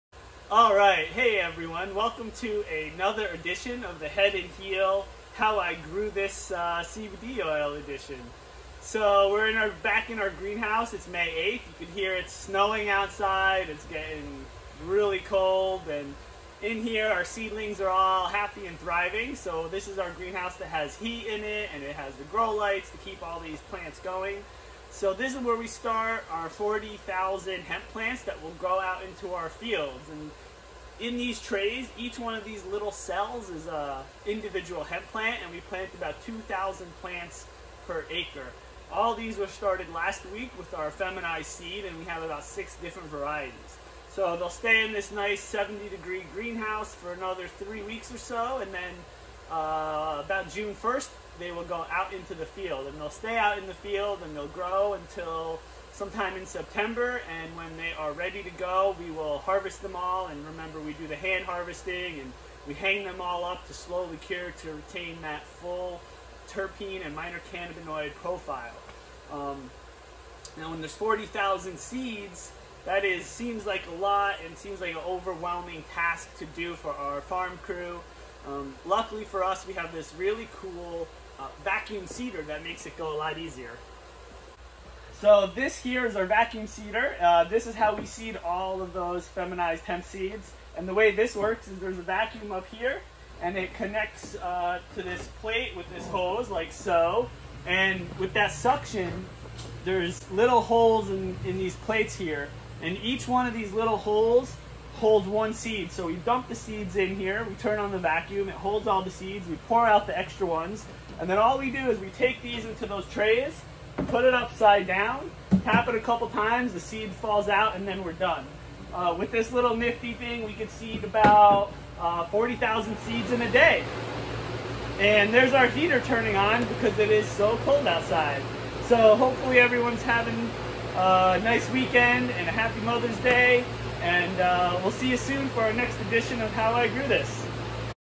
Starting Hemp Seeds for CBD
Today we bring you into our greenhouse and show you how we start 40,000 hemp plants in one day!